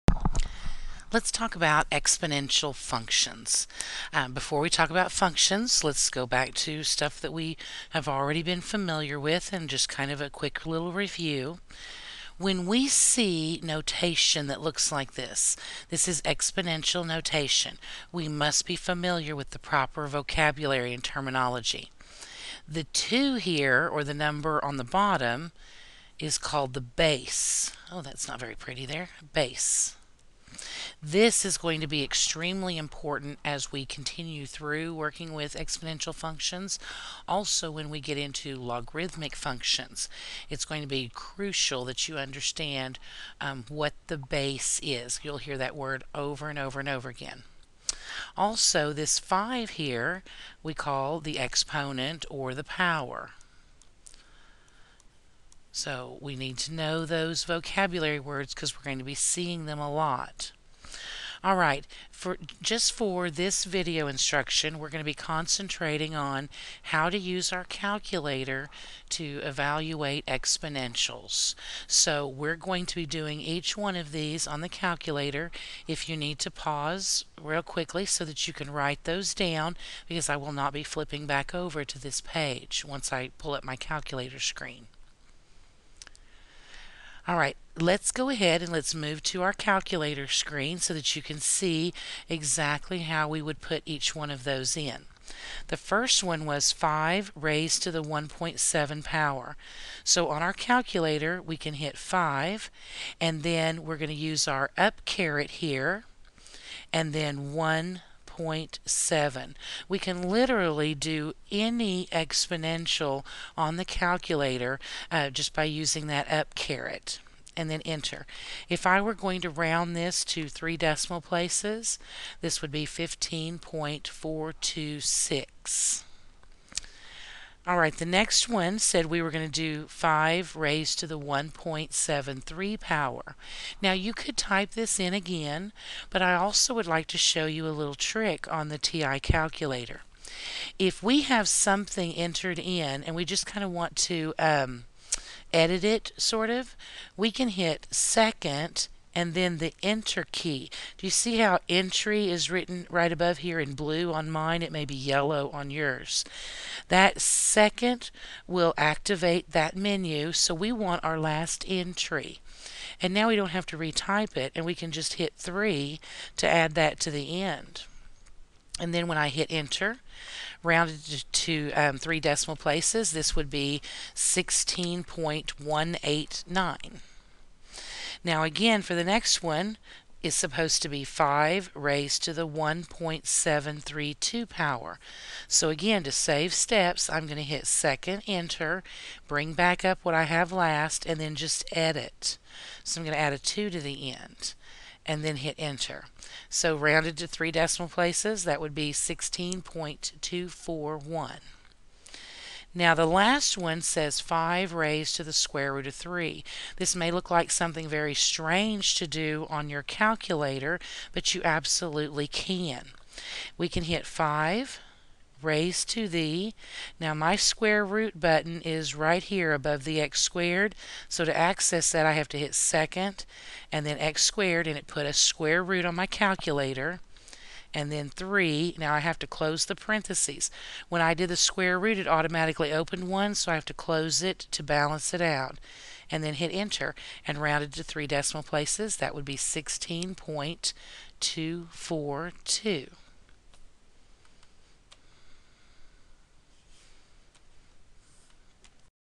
Exponents on a Calculator 6.3
How to use a TI-83/84 to evaluate exponents.